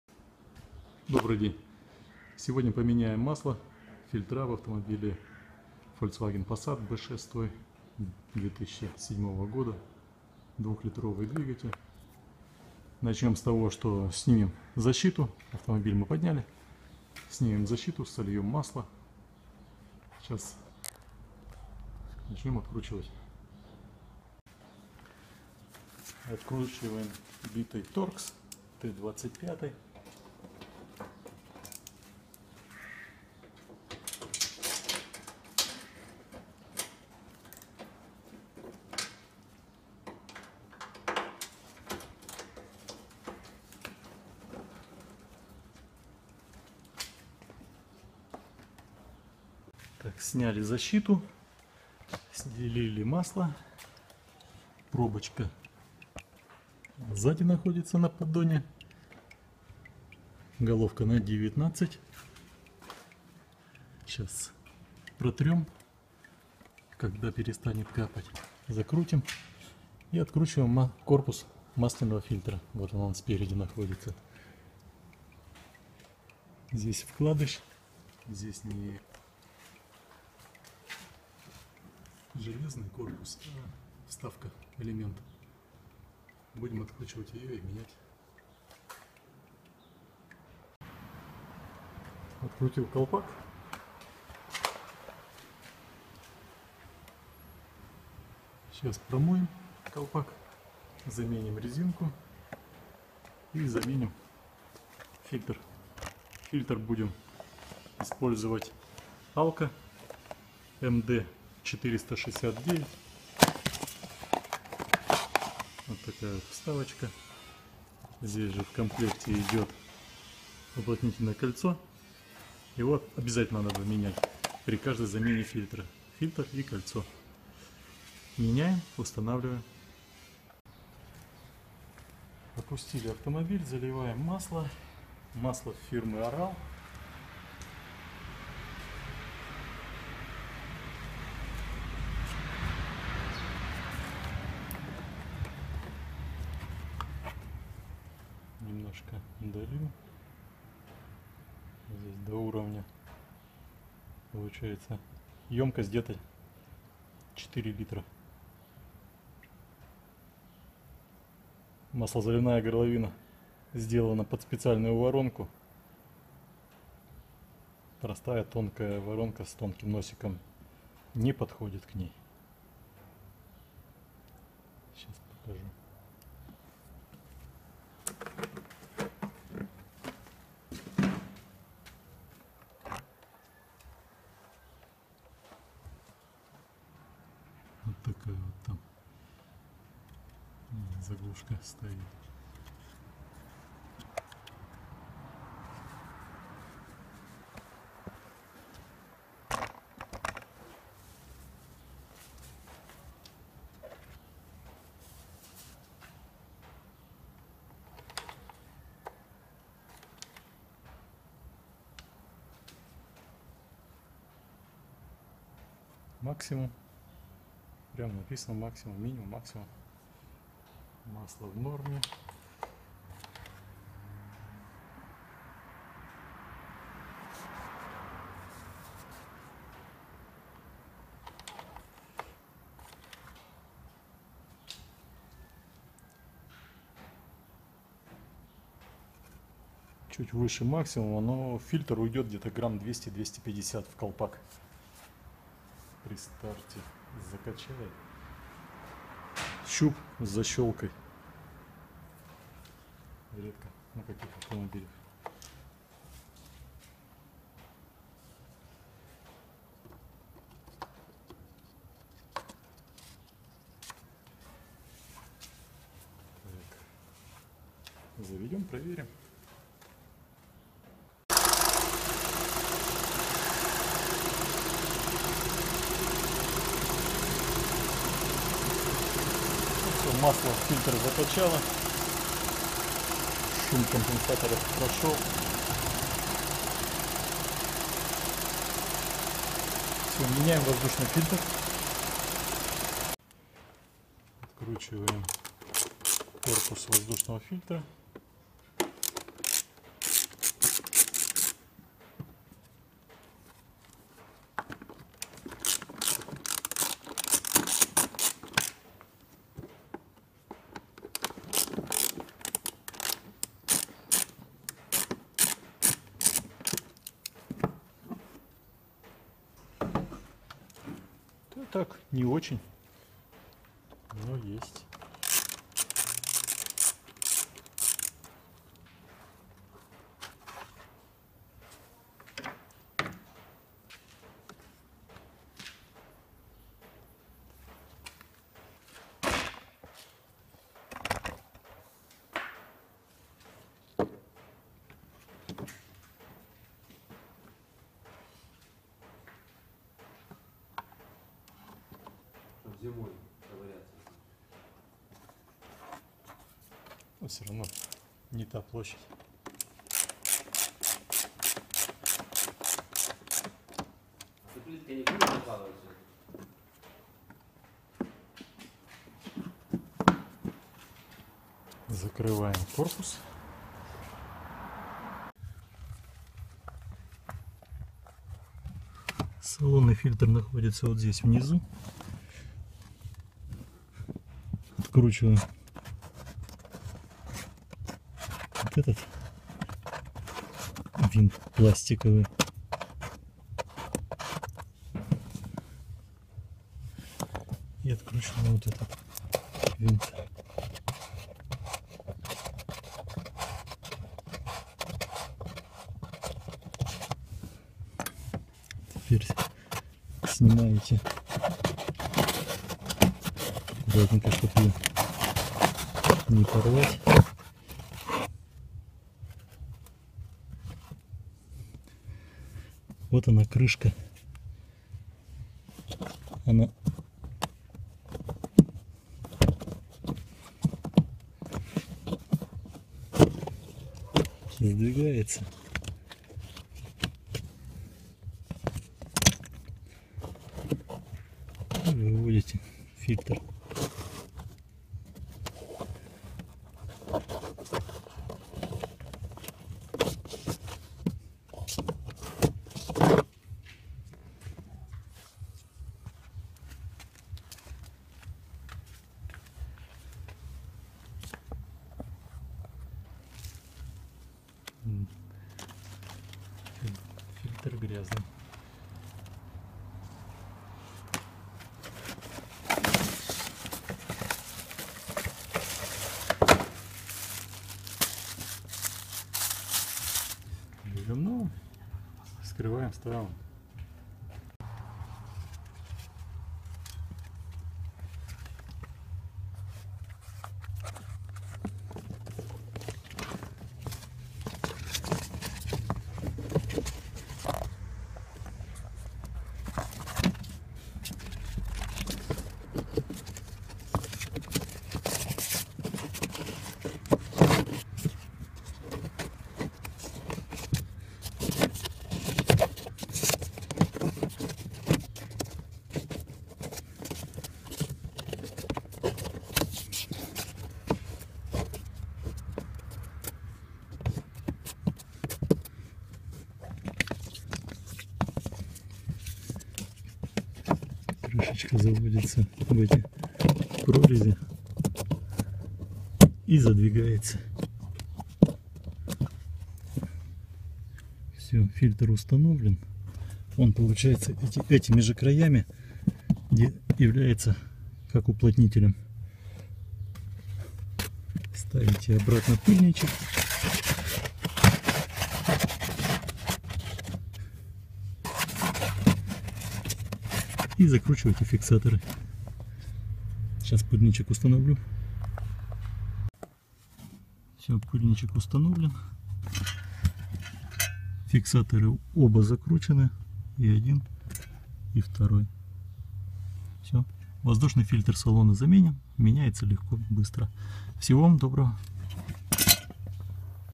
Замена масла и фильтров на фольксваген пассат Б6 2007 года Volkswagen Passat B6
Меняем масло и фильтра на фольксваген пассат Б6  2007 года  Volkswagen Passat  B6
всего вам наилучшего.